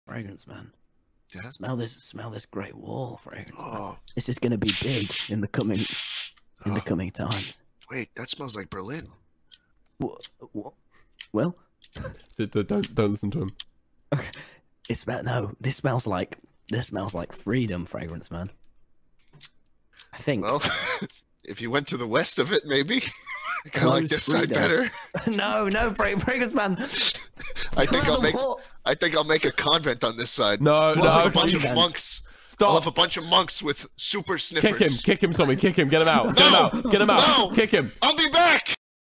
Smells like Berlin - OC animatic
All the characters in this belong to me-

Also yes I used scratch to animate